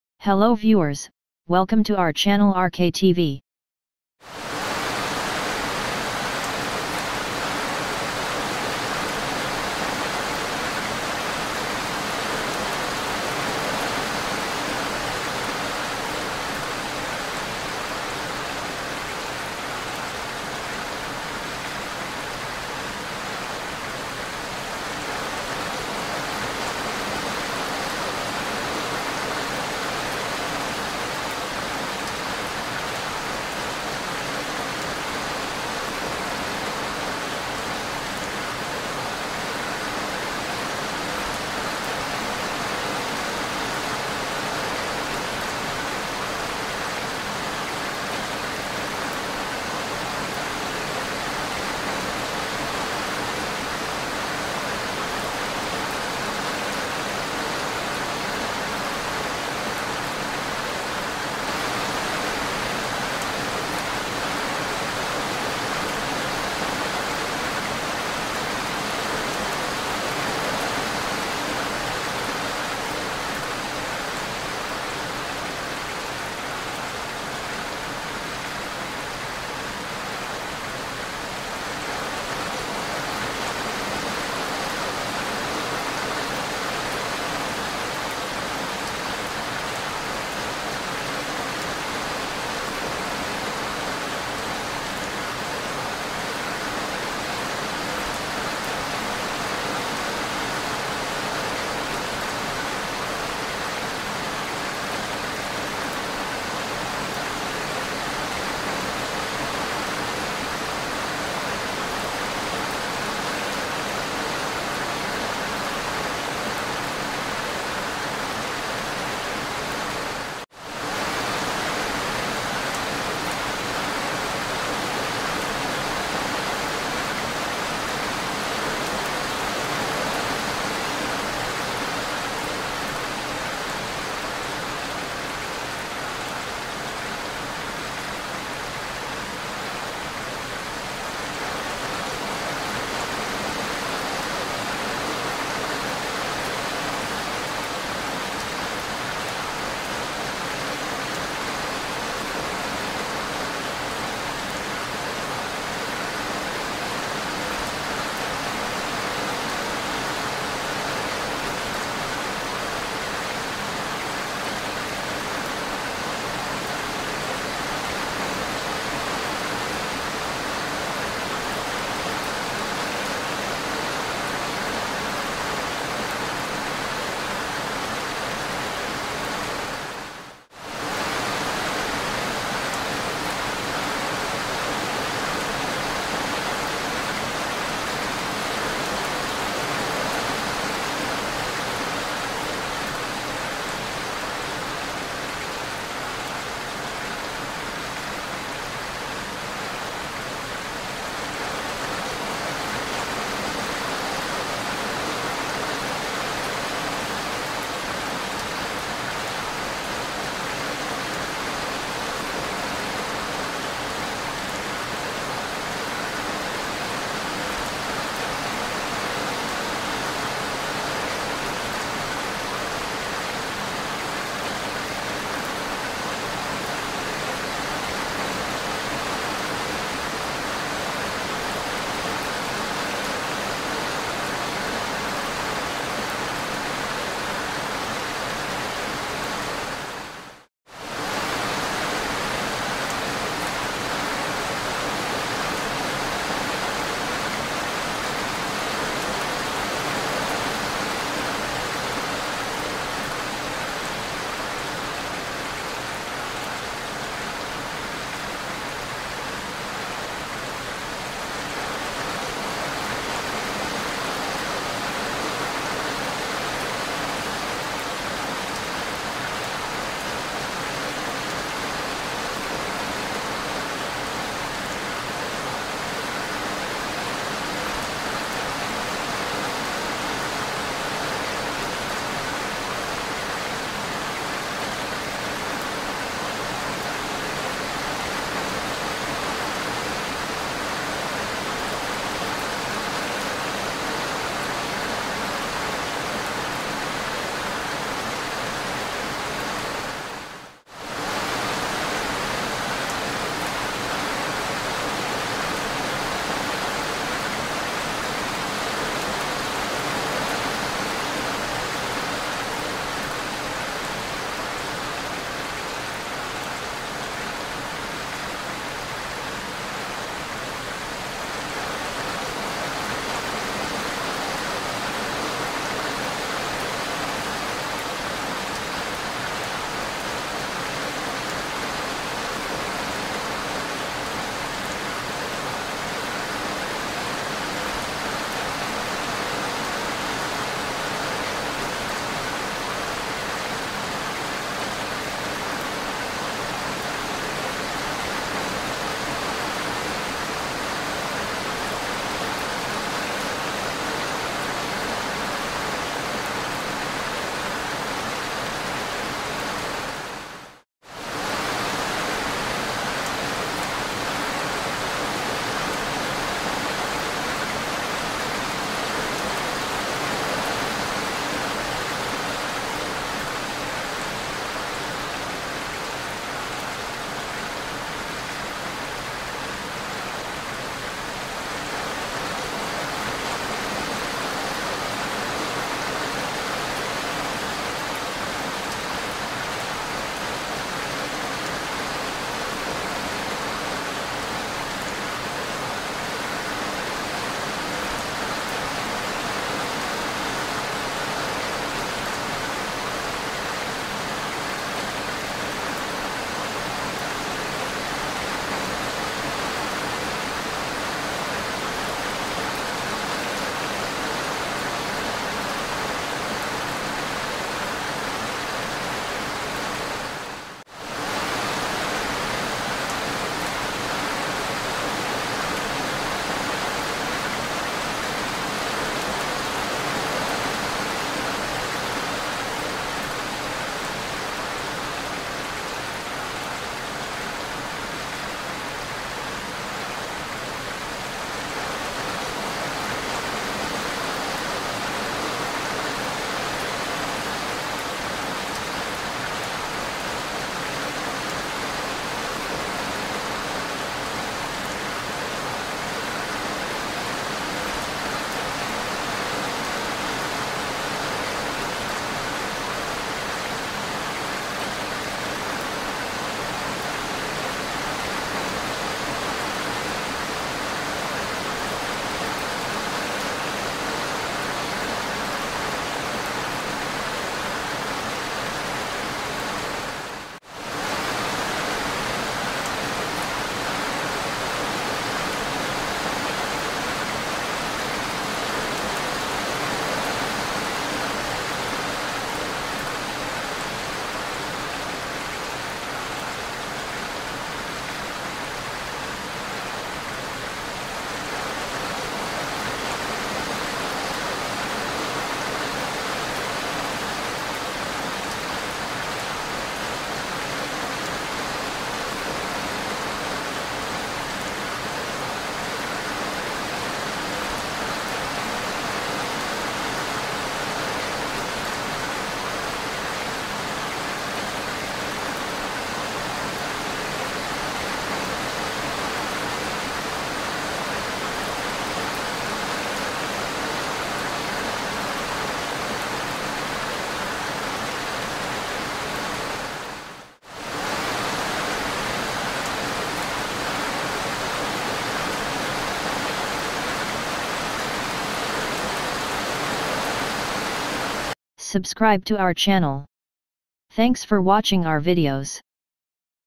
Rain on Car Sound BLACK SCREEN White Noise for Sleeping, Studying, Relaxing,
Rain on Car Sound | BLACK SCREEN | White Noise for Sleeping, Studying, Relaxing, Meditation

Use this rain on car windshield sound to sleep, study, meditate, or relax. This video has a black screen to save battery, data, and provide a dark room if using for sleeping.